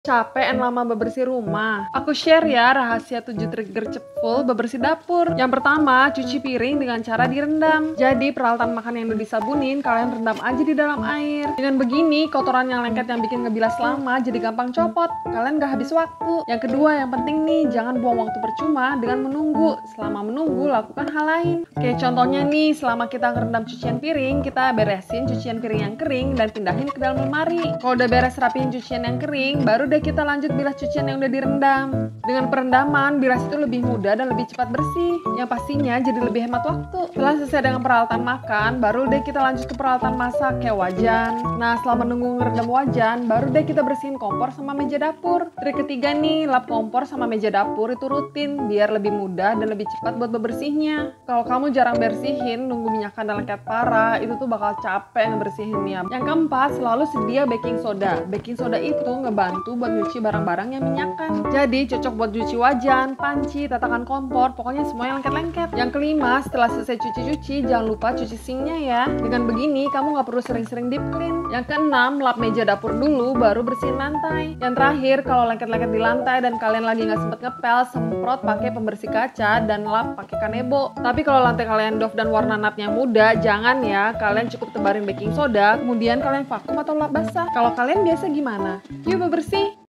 RAHASIA 7 TRIK GERCEP BEBERSIH DAPUR!! KERJA EFEKTIF DAN EFISIEN BIAR GA HABIS WAKTU!
CAPEK N LAMA BEBERSIH RUMAH?! CEK RAHASIA 7 TRIK GERCEP FULL BEBERSIH DAPUR! CUSSSS SENGGOL SEMUA BESTIES BIAR MEREKA GA TUA DI DAPUR! 😁

Biar ga makan waktu n habis tenaga, cobain 7 trik ini ya 😍

1. Cuci piring dengan teknik perendaman
Kalo sink kalian ga double kalian bisa pakai baskom ya buat ngerendem cucian piringnya.
Pokoknya sebelum direndam, sink dan baskom dicuci dulu ya. Dengan teknik perendaman, segala kotoran yang lengket2 dan biasa ngeras di piring dan peralatan makan jadi lebih cepat copot dan lunak. Dengan begitu, ngebilasnya jadi lebih cepat.

2. Jangan habiskan waktu menunggu, selama menunggu lakukan kerjaan lain.
Contoh kayak selama ngerendam cucian piring, kita rapihkan cucian kering. Atau selama ngerendam wajan kita bersihin kompor dan meja dapur. Jadi kerjaan lebih cepat selesai klo begini.

3. Lap kompor dan meja dapur secara rutin. Dengan begini bebersih jadi lebih mudah dan cepat. Selain itu ga perlu sering2 deep clean juga 🤩 hemat waktu dan tenaga deh

4. Selalu sedia BAKING SODA. Untuk barang2 yang minyakan parah kayak wajan, panci, tatakan kompor cusss dah taburin baking soda yaa.

5. Setelah selesai cuci piring, jangan lupa cuci sink ya. Dengan begitu ga perlu sering2 deep clean juga dan ga sampe kotor jelek sinknya 🫰

6. Lap meja dapur dulu baru bersihkan lantai. Biasa ada kotoran yang suka jatuh dari meja2 tuh, klo kita urusin lantai dulu nanti kita kerja 2x pas kotorannya jatuh. Usahakan semua gerakan efektif dan efisien biar ga buang waktu ya 🥰

7. Klo ga sempet ngepel tapi lantai lengket, ada 2 cara nih

Untuk lantai glossy, vinyl dengan nat warna gelap cusss semprot aja pembersih kaca dan lap pake kanebo

Untuk lantai doff dengan nat warna cerah tabur baking soda dikit aja, sikat ringan buat nebar si baking soda, lalu lap basah atau vakum 🥰

Klo kalian biasa gimana? Yuk bebersih!